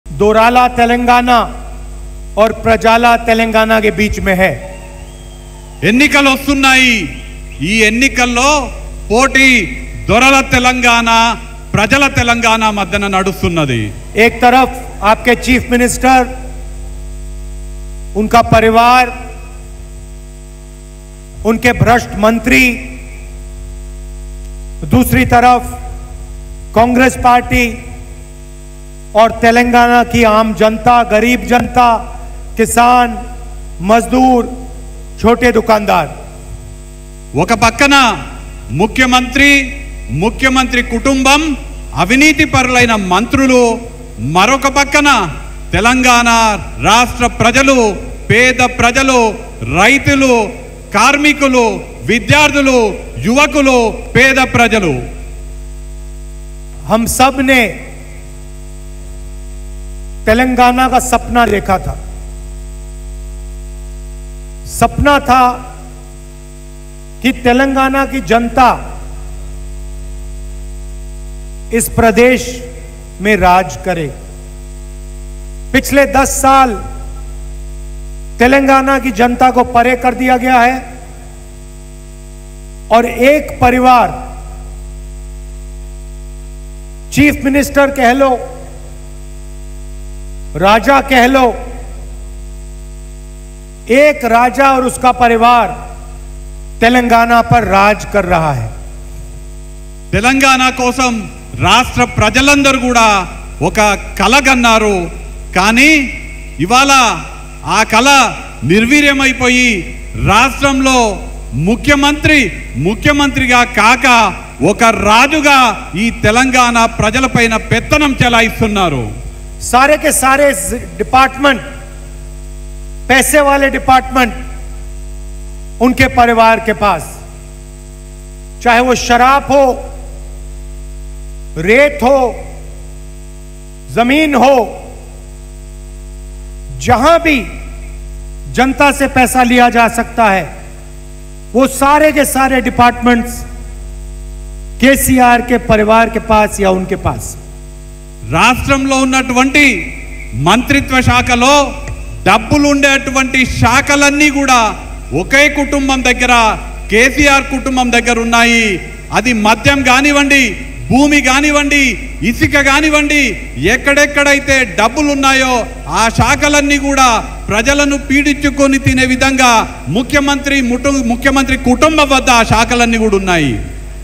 Rahul Gandhi Comments On CM KCR | కేసీఆర్ దగ్గరే ఆదాయం వచ్చే శాఖలు | ZEE Telugu News
కేసీఆర్ దగ్గరే  ఆదాయం వచ్చే శాఖలు | Rahul Gandhi Comments On CM KCR | ZEE Telugu News